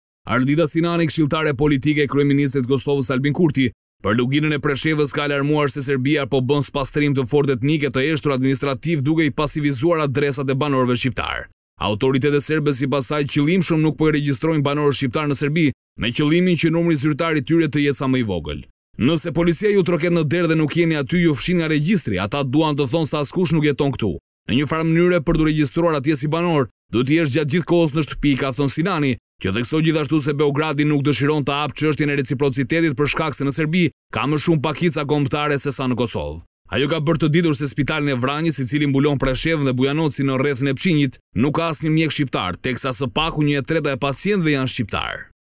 Në Luginë të Preshevës po bëhet spastrim i fortë etnik
Ardita Sinani, këshilltare politike e kryeministrit të Kosovës, Albin Kurti, për Luginën e Preshevës, ka alarmuar se Serbia po bën spastrim “të ftohtë etnik”, e të “heshtur administrativ”, duke i pasivizuar adresat e banorëve shqiptarë.
Autoritetet serbe, sipas saj, qëllimshëm nuk po i regjistrojnë banorët shqiptarë në Serbi, me qëllimin që numri zyrtar i tyre të jetë sa më i vogël.
“Nëse policia ju troket në derë dhe nuk jeni aty, ju fshijnë nga regjistri. Ata duan të thonë se askush nuk jeton këtu. Në njëfarë mënyre, për t’u regjistruar atje si banor, duhet të jesh gjatë gjithë kohës në shtëpi”, ka thënë Sinani qe theksoi gjithashtu se Beogradi nuk dëshiron ta hapë çështjen e reciprocitetit për shkak se “në Serbi ka më shumë pakica kombëtare sesa në Kosovë”.
Ajo ka bërë të ditur se “spitalin e Vranjës, i cili mbulon Preshevën dhe Bujanocin, në rrethin e Pçinjit, nuk ka asnjë mjek shqiptar, teksa së paku një e treta e pacientëve janë shqiptarë”

𝐀𝐮𝐭𝐨𝐫 : NTV